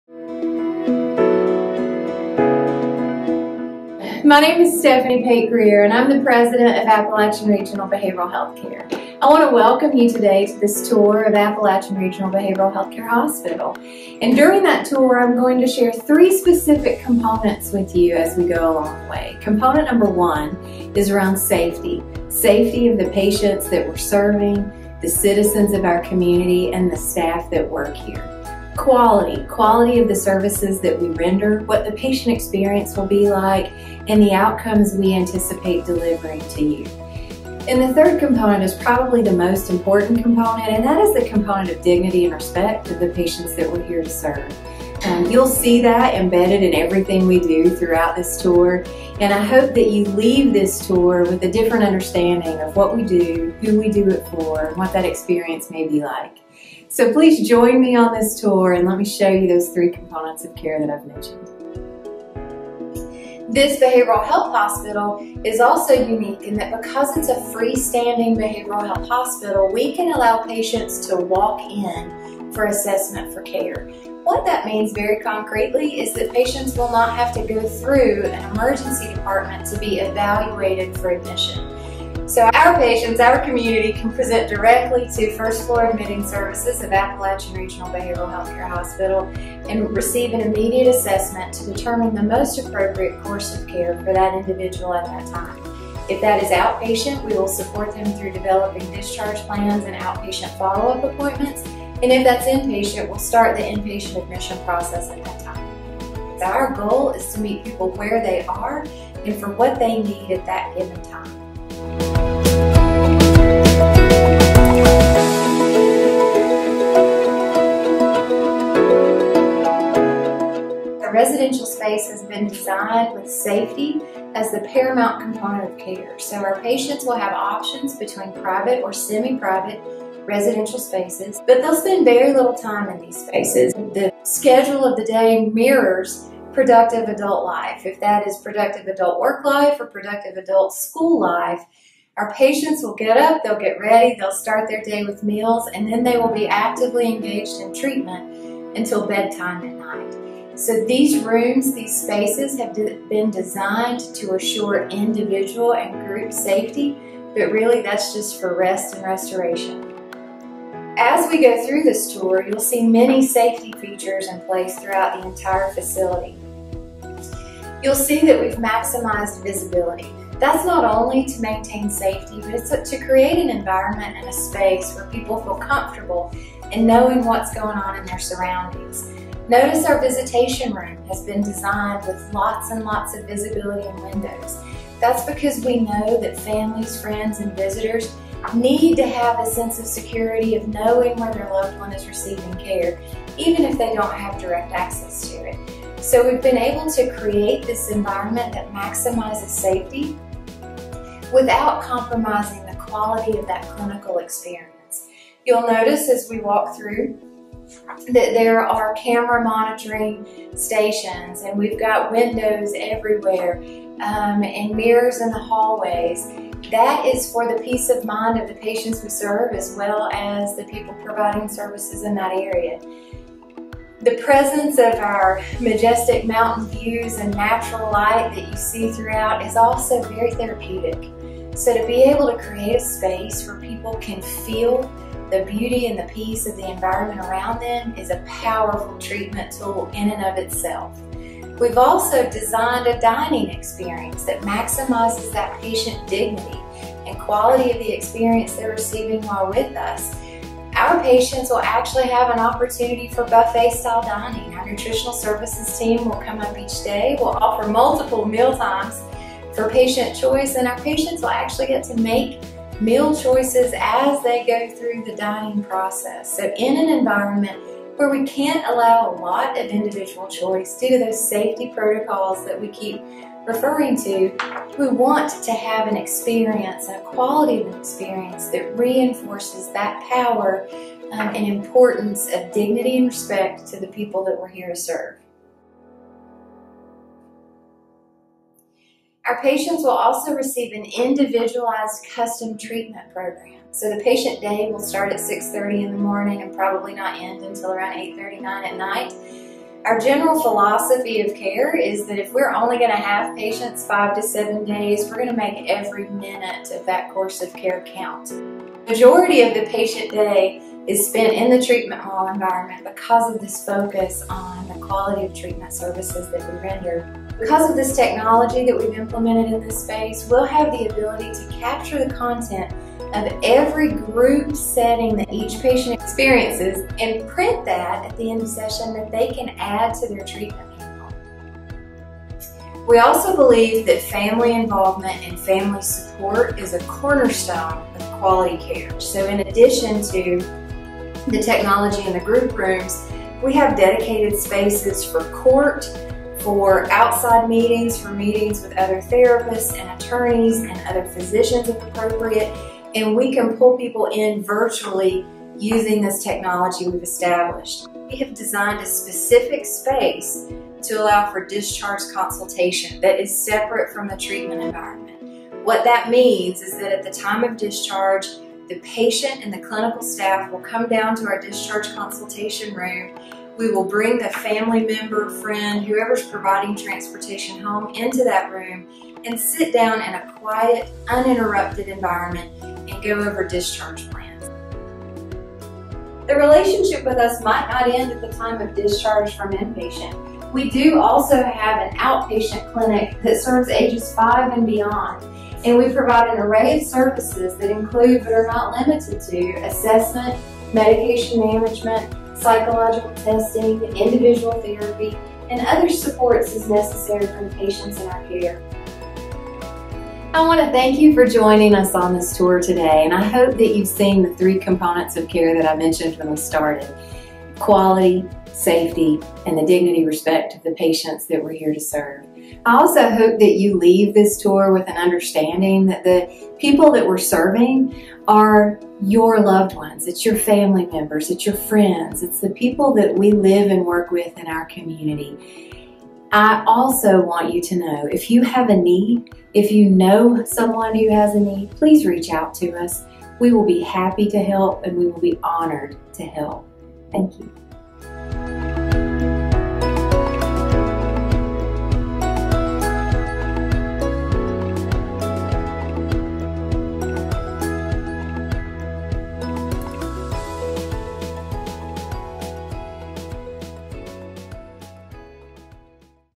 Virtual Tour of the Appalachian Regional Behavioral Health Hospital in Linville, NC
Step inside a new, top-tier behavioral health hospital.